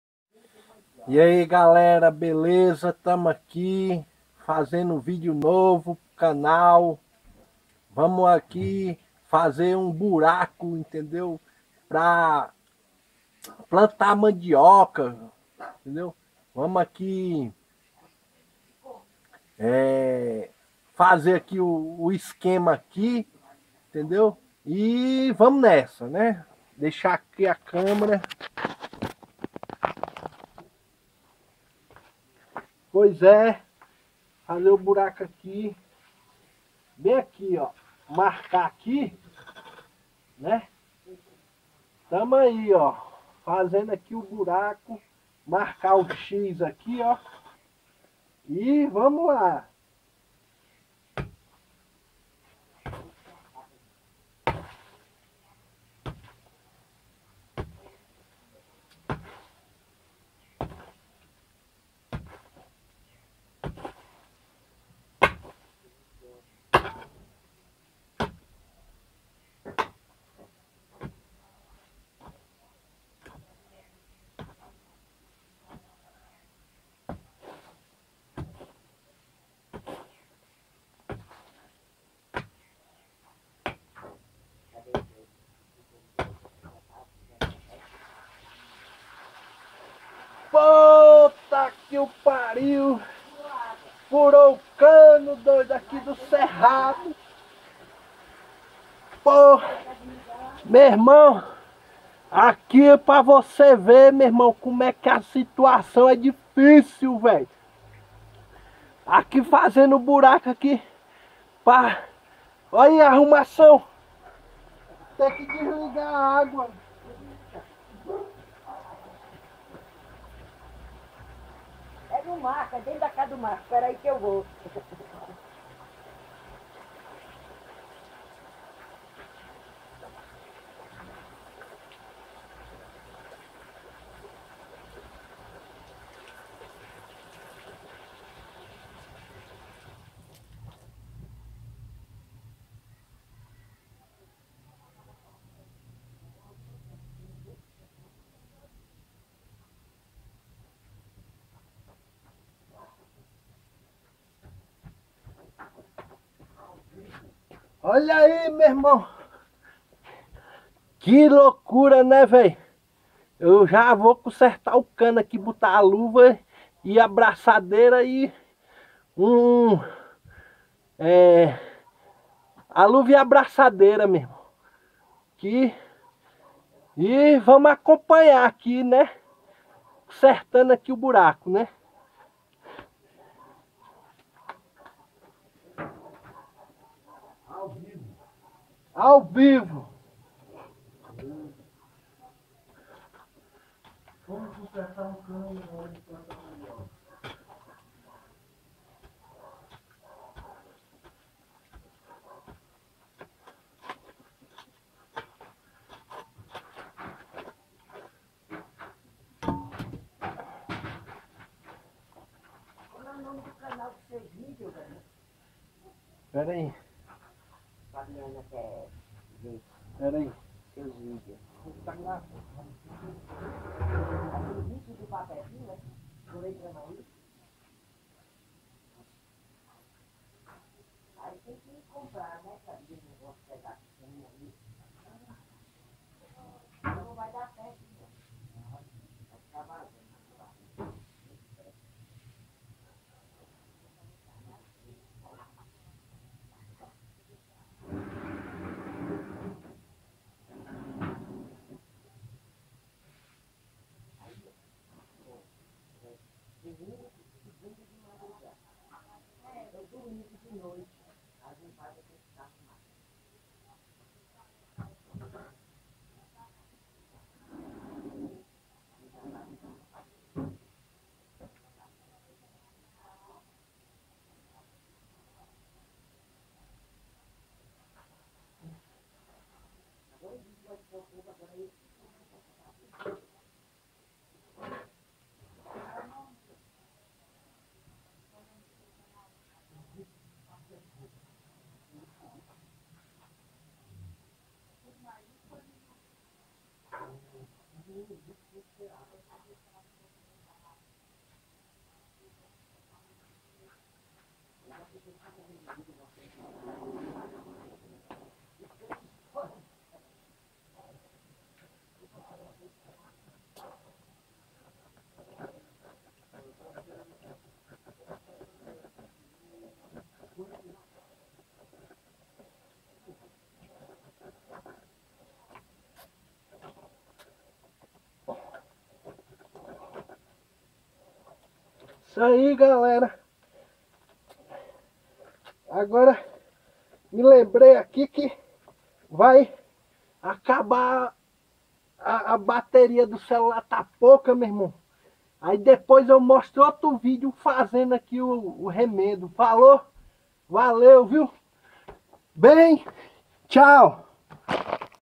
Estorando cano sem querer/Unintentionally draining a pipe
Cavando buraco para plantar mandioca e um assidente para encanador ver